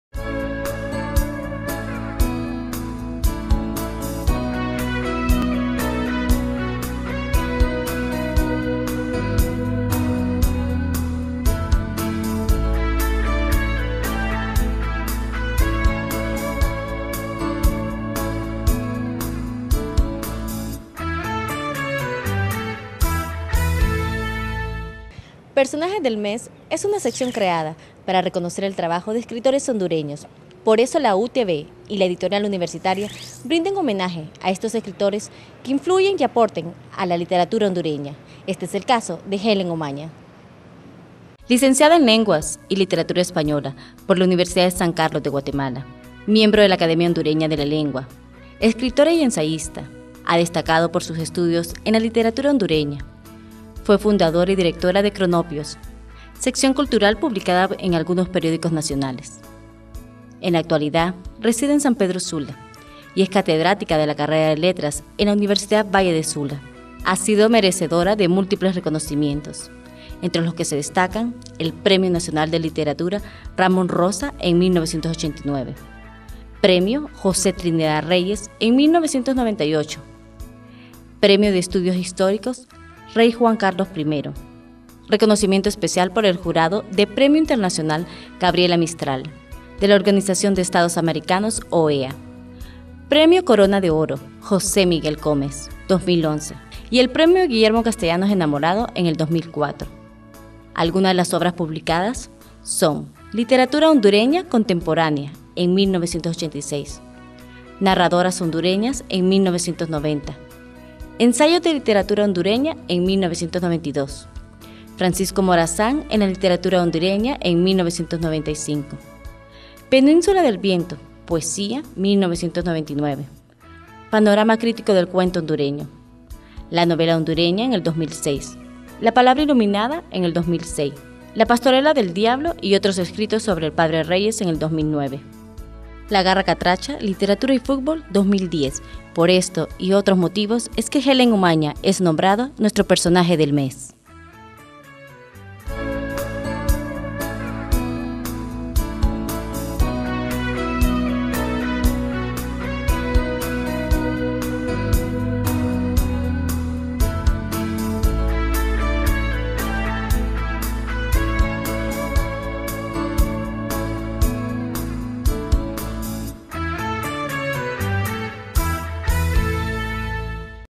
UTV: Personaje del mes: Helen Umaña (Escritora)
Una de las pocas escritoras hondureñas que ha enfocado su labor literaria hacia el ensayo y la crítica literaria. Destacada por su incansable labor en el rescate de obras que se hallaban en el olvido y que su vida la dedicó al estudio crítico e historiográfico de las letras.